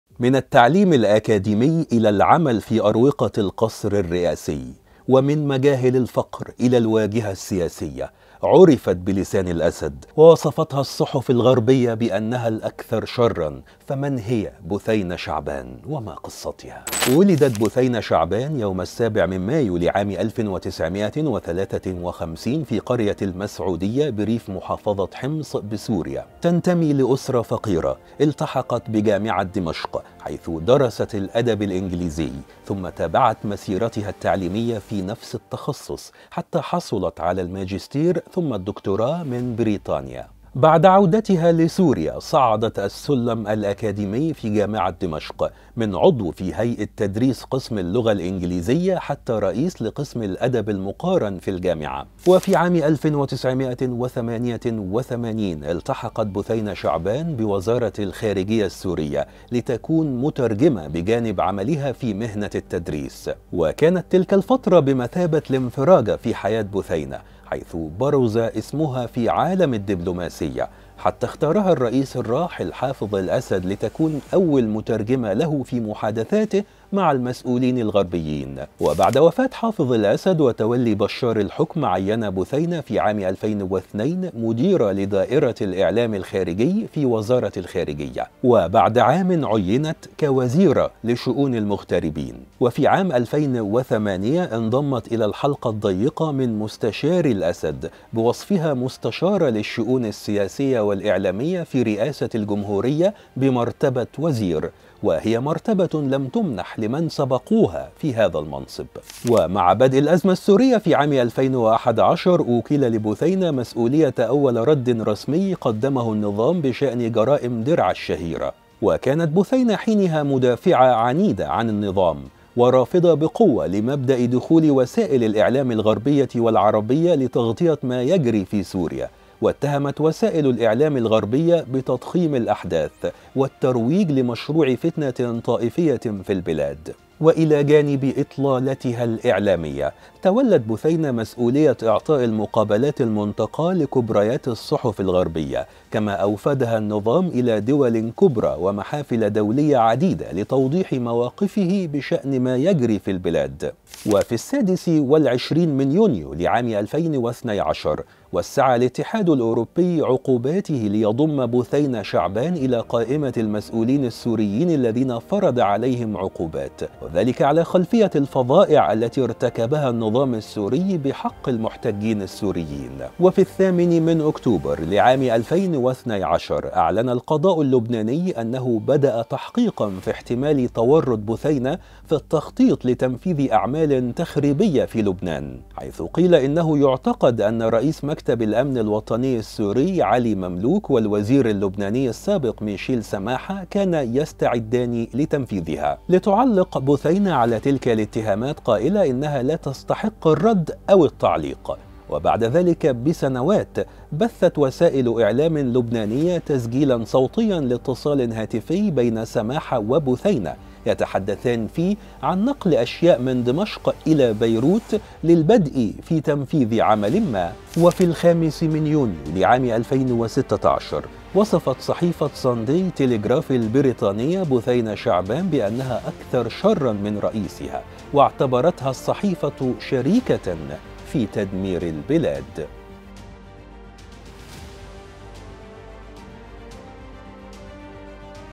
من هي بثينة شعبان المترجمة الخاصة بحافظ الأسد ولسان النظام؟ ولماذا وصفتها الصحف الغربية بالأكثر شرا؟
من التعليم الأكاديمي إلى العمل في أروقة القصر الرئاسي ومن مجاهل الفقر إلى الواجهة السياسية..عرفت بلسان الأسد ووصفتها الصحف الغربية بأنها الأكثر شرا, فمن هي بثينة شعبان وما قصتها؟